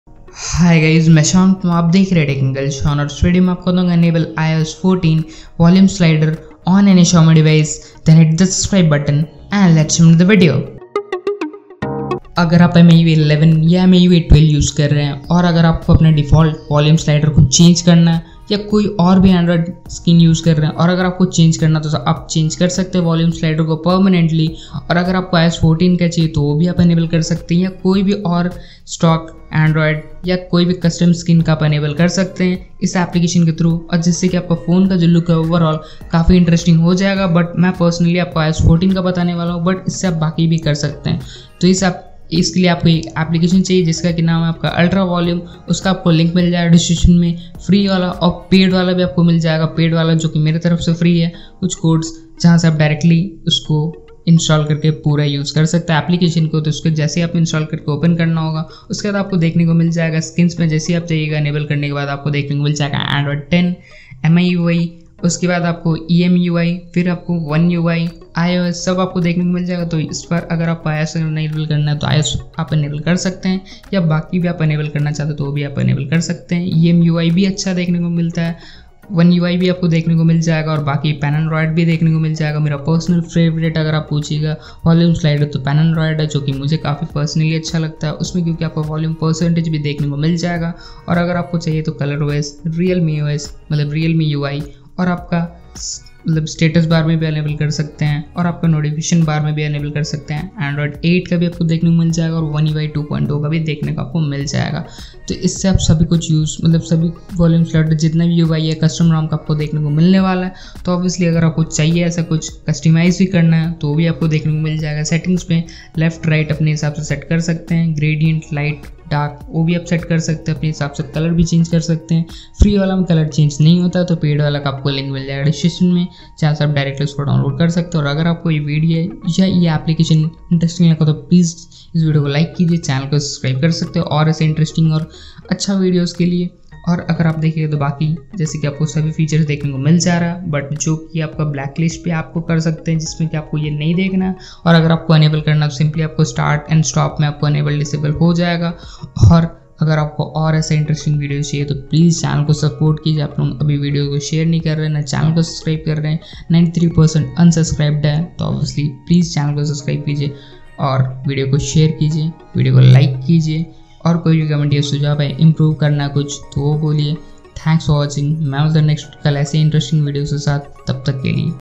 Enable iOS 14 Volume Slider On Any Xiaomi Device | Redmi Note 8 Pro/Poco X2
In this video I will show you Enable iOS 14 Volume Slider On Any Xiaomi Device | Redmi Note 8 Pro/Poco X2

If You Want To Change Your Default Volume Slider Or Try Some New Volume Sliders Like Android 11,One Ui 2,Paranoid And Many More.